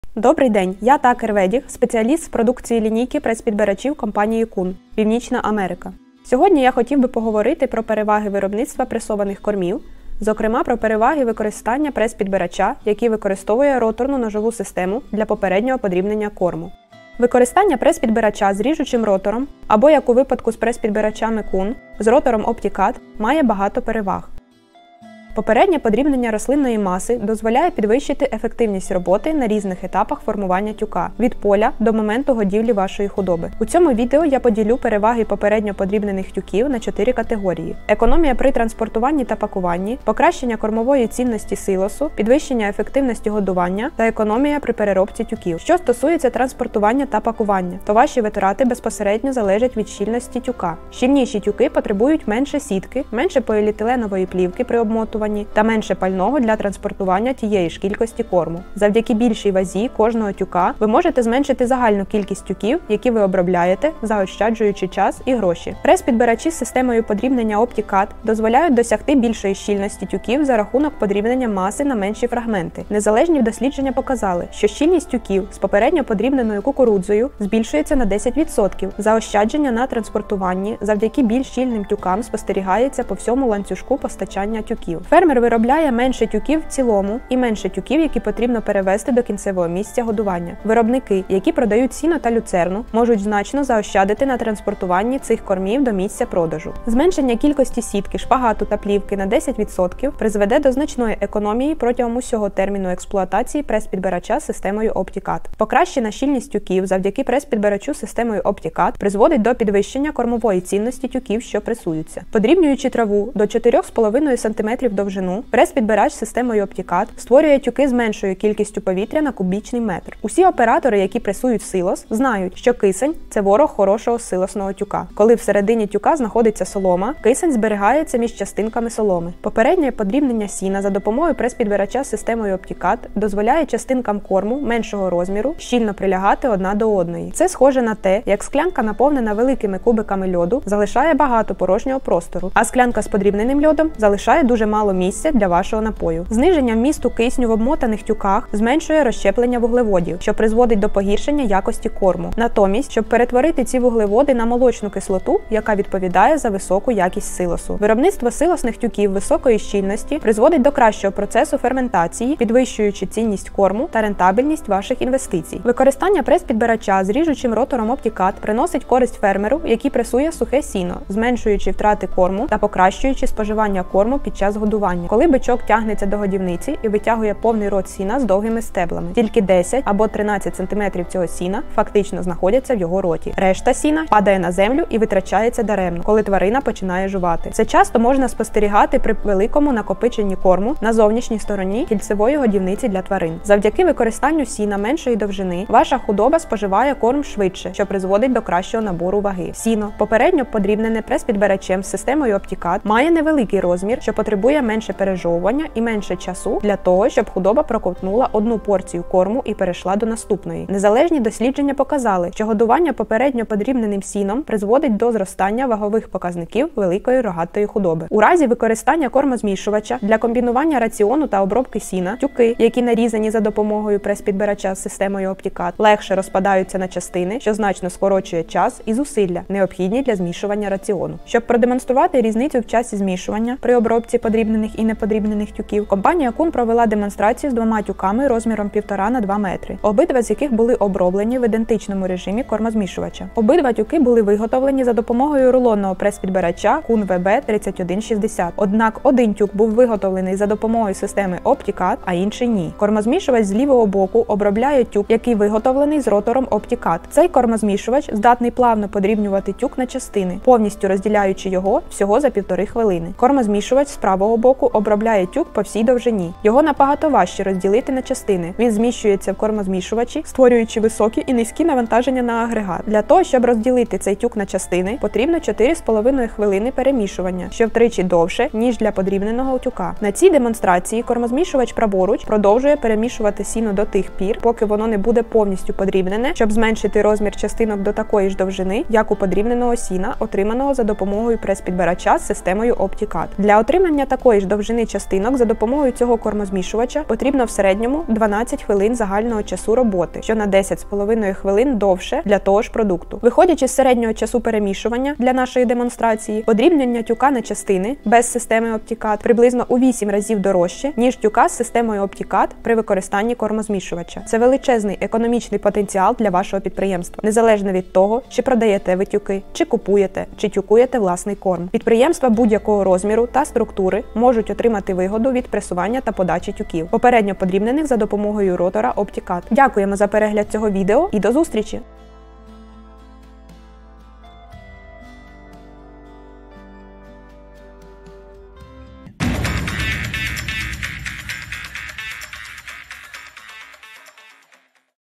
Переваги попередньо подрібненого фуражу у тваринництві
У цьому відео ви дізнаєтеся, як тюки, виготовлені з попередньо подрібненого фуражу, забезпечують значні переваги для вашого прибутку.

Це включає в себе економію на транспортуванні та пакуванні, покращену кормову цінність силосу, підвищену ефективність годівлі та економію часу при обробці тюків.